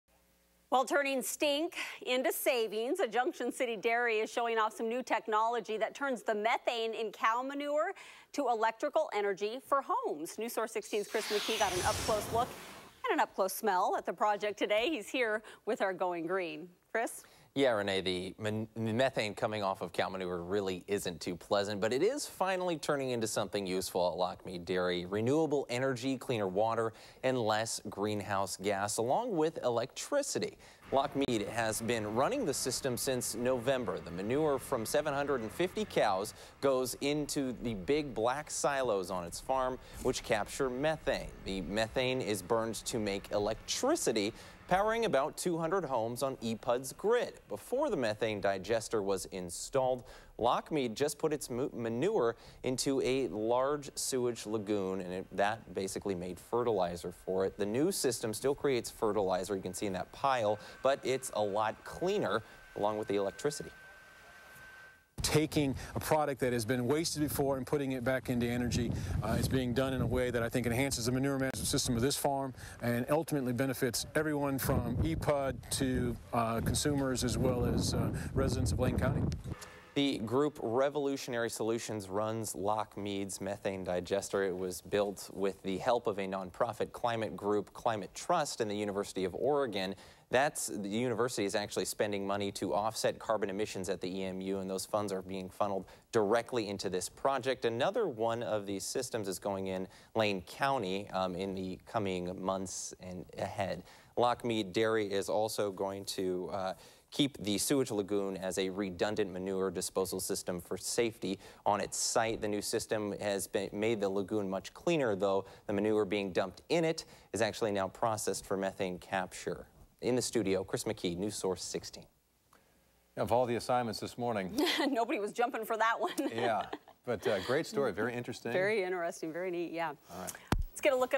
Lochmead Farms Digester converts manure into energy
Lochmead Dairy has a new way to handle cow manure with the installation of a digester. Methane in cow manure is turned into energy that can power about 200 homes on the EPUD grid. Chris McKee from KMTR NewsSource 16 reports.